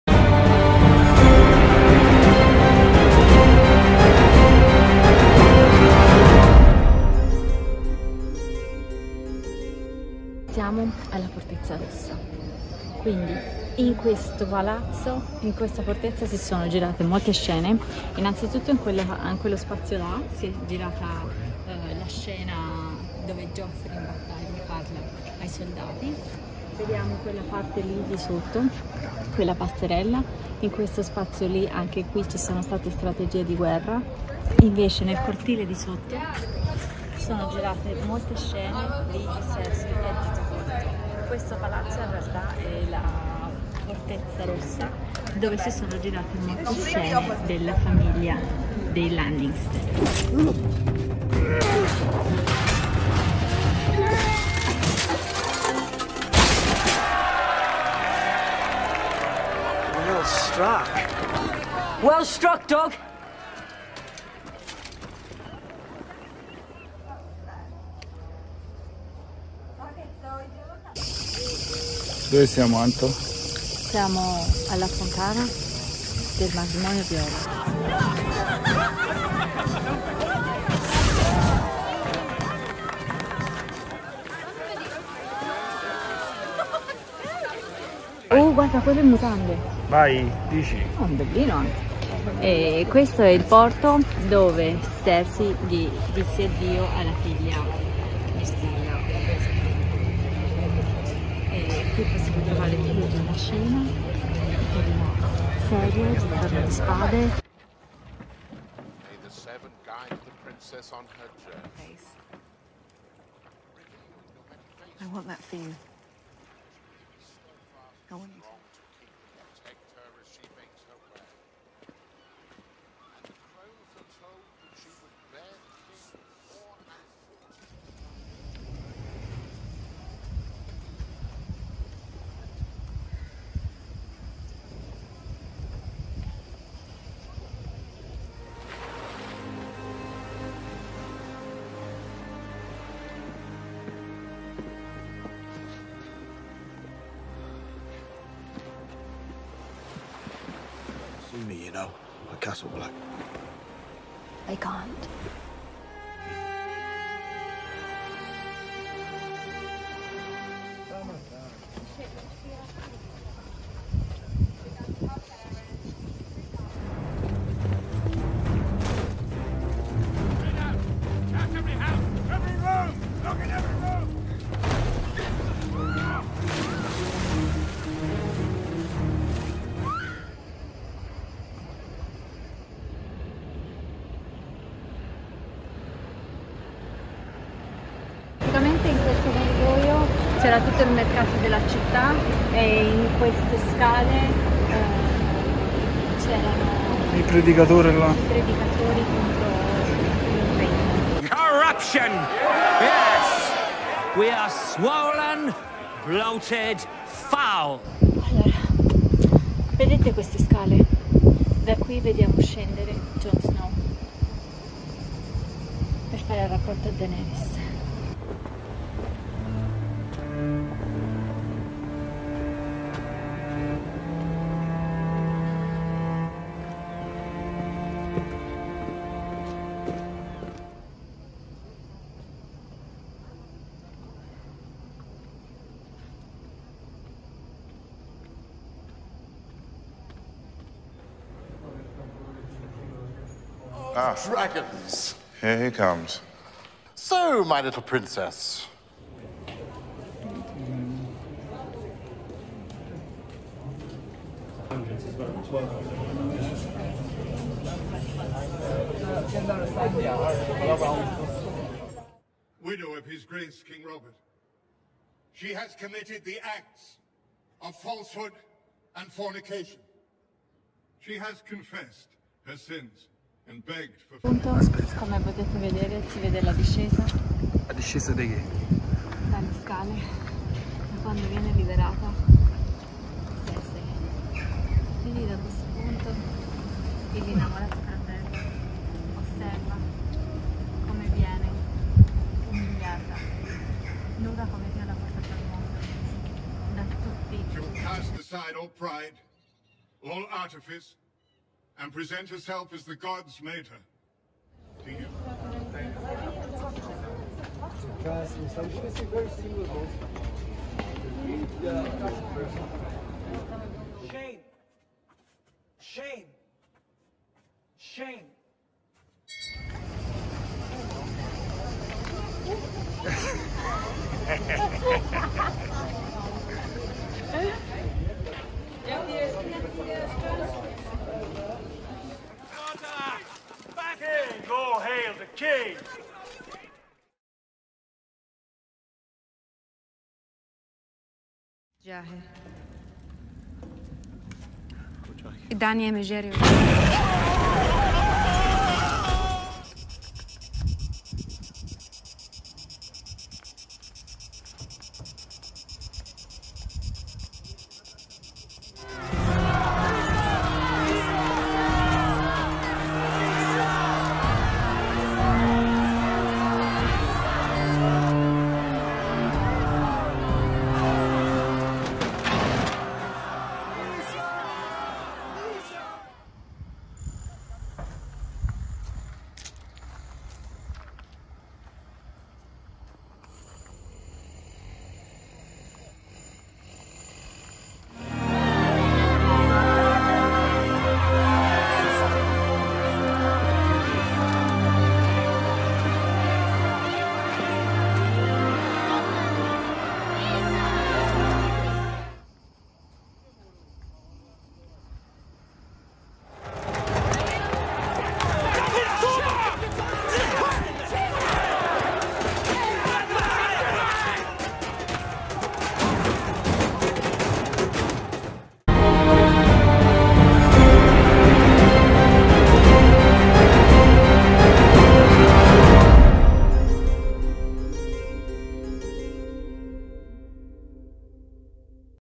Croazia 2021 - Games of Thrones
Croatia, Games of Thrones location.
Dubronik, Split and Fortress Klis.